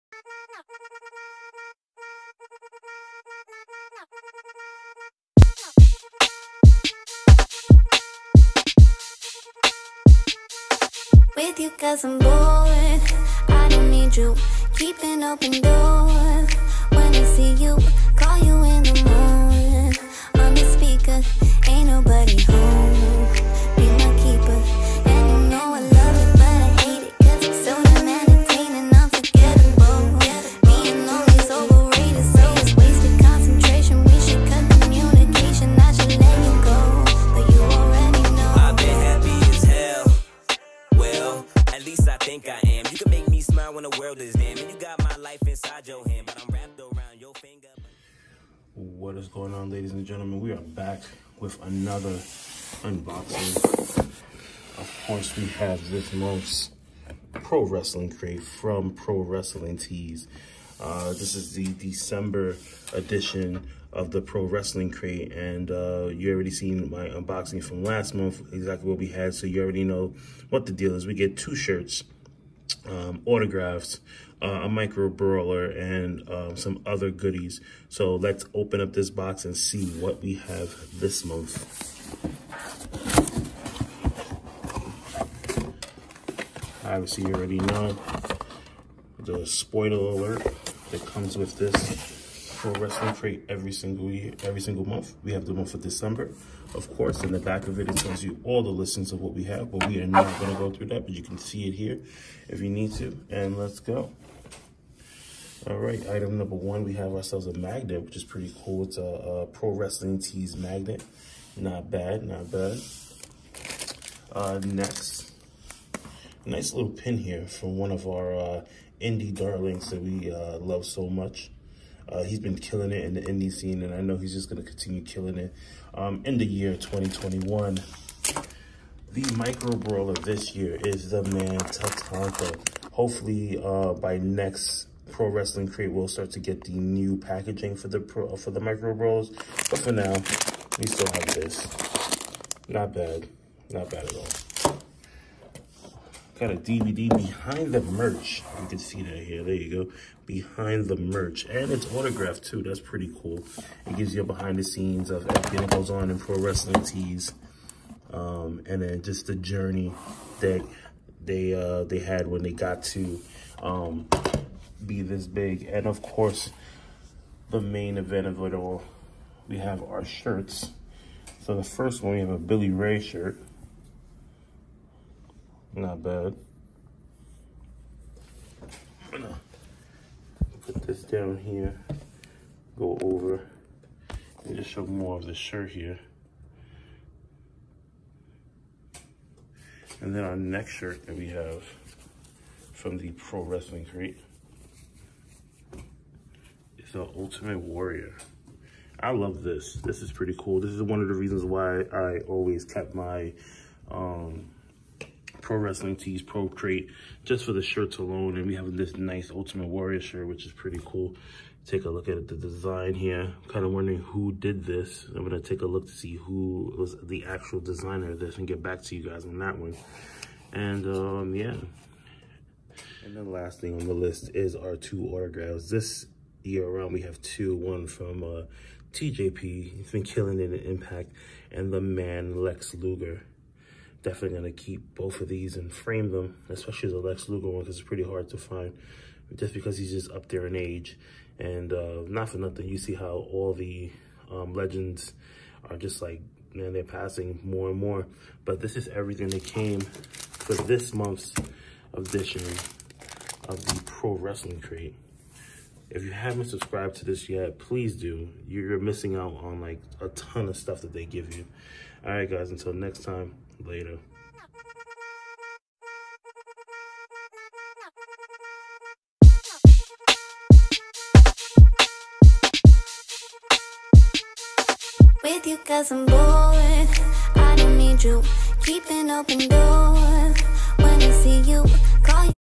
DECEMBER PRO WRESTLING CRATE
Welcome to another unboxing this months pro wrestling crate is bigger and better than last month so let’s take a look